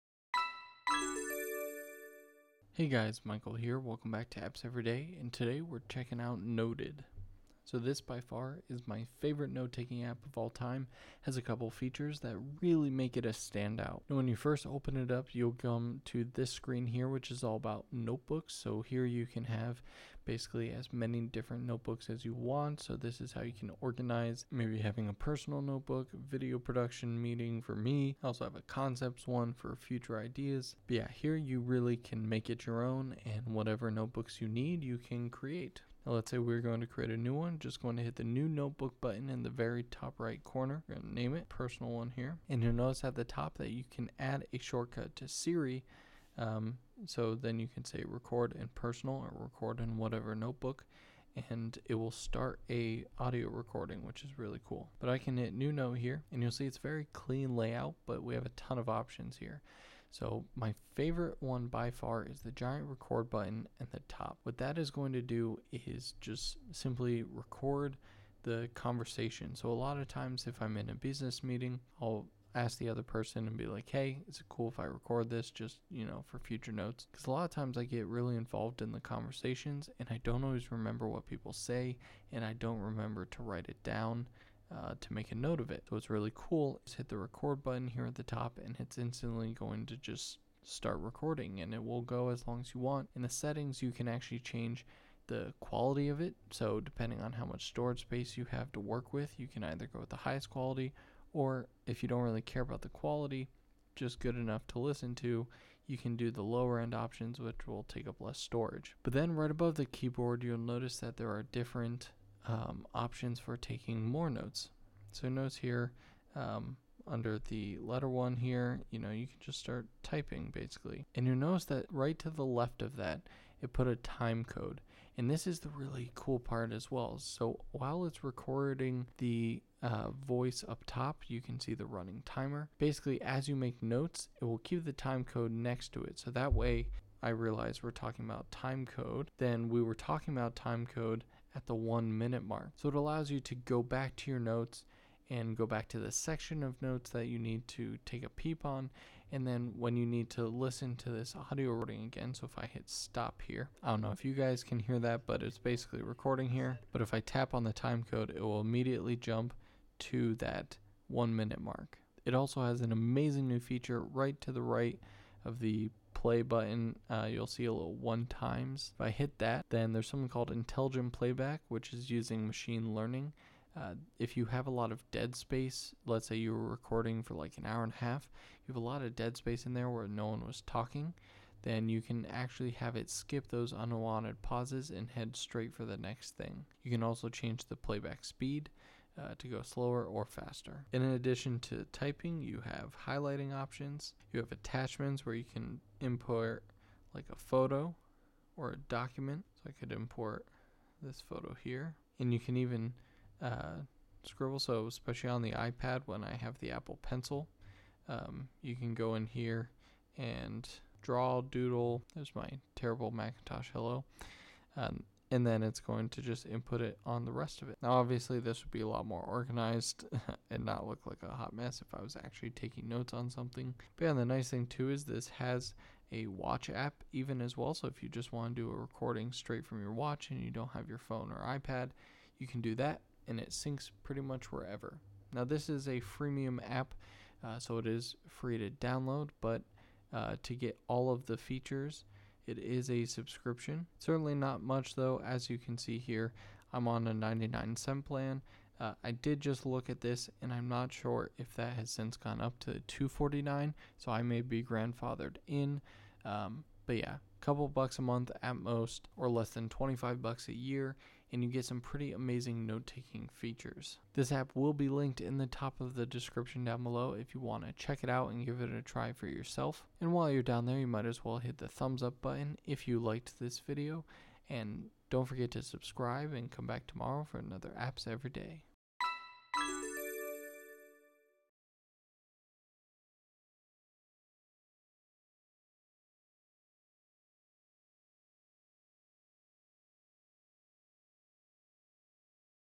The BEST Note Taking App, Noted | Apps Every Day #39
With new features involving machine learning, this is truly the BEST note taking app out there. Check it out